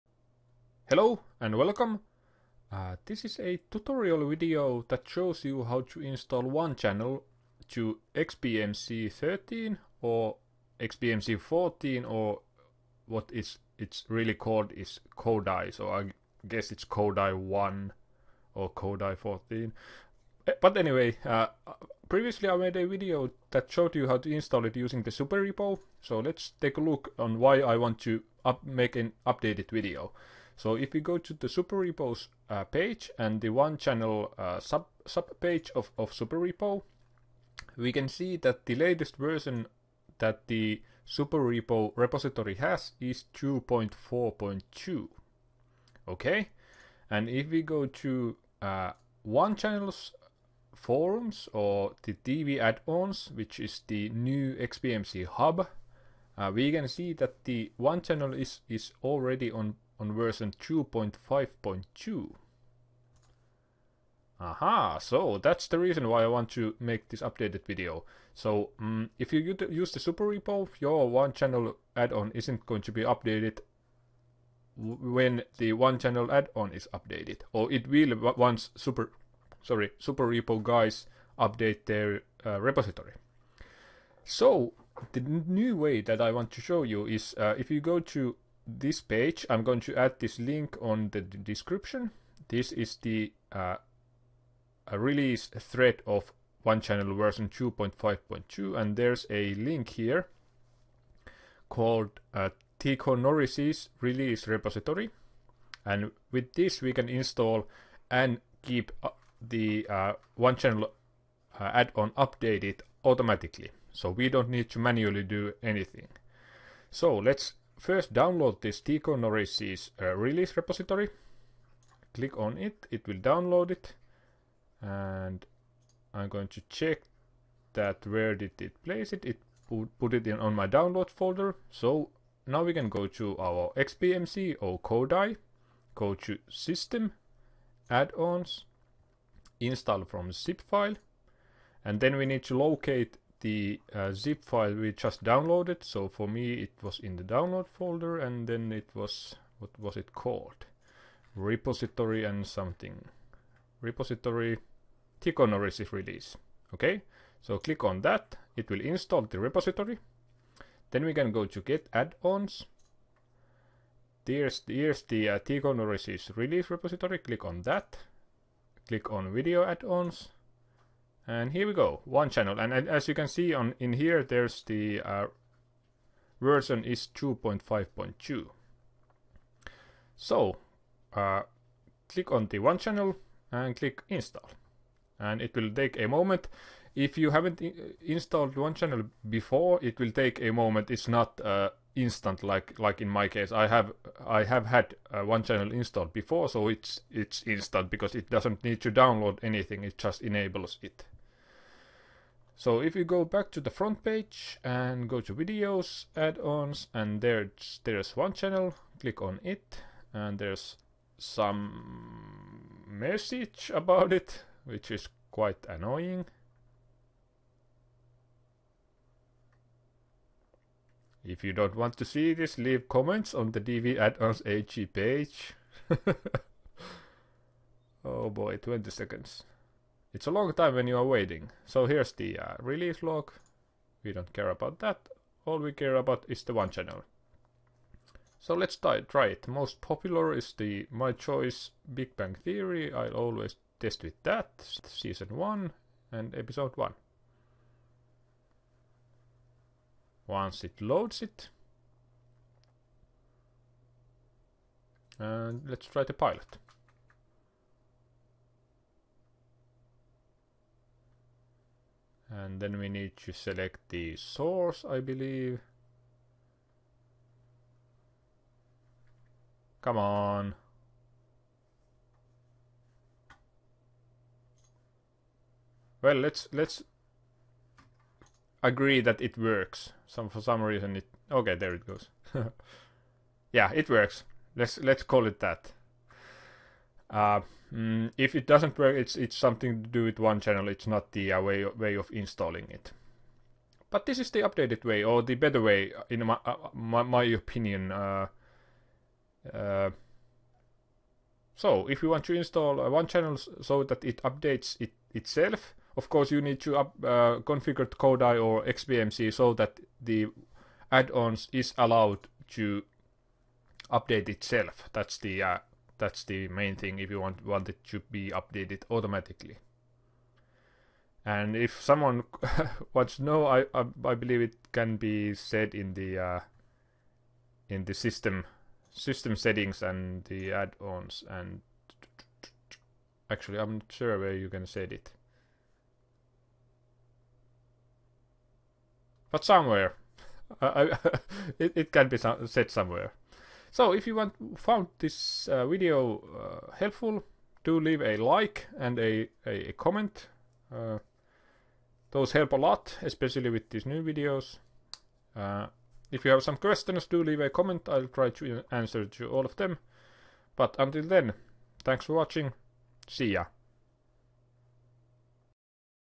**Updated** Install 1-Channel for XBMC/Kodi [10/2014] [Win7]
On this video I'll show how to install 1Channel for XBMC-Kodi. This way will keep your 1Channel updated automatically when new versions of 1Channel is released.